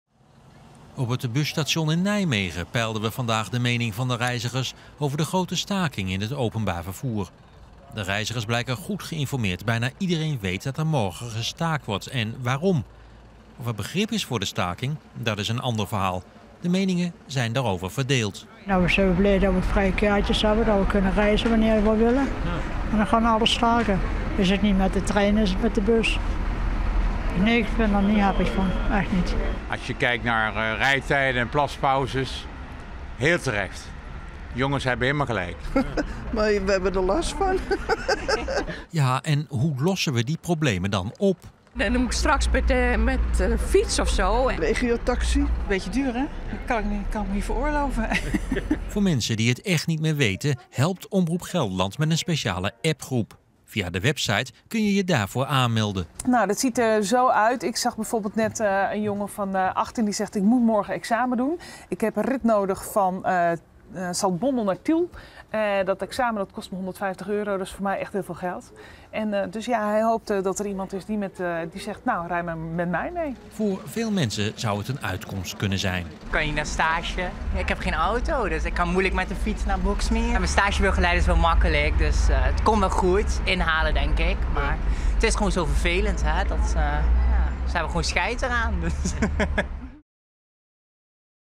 Achterhoek gespaard bij ov-staking; hoe zit het verder?
- De landelijke stakingen in het openbaar vervoer vanaf woensdag zullen de Achterhoek niet zo hard treffen. Vervoerder Arriva zal daar gewoon bus- en treinvervoer blijven verzorgen.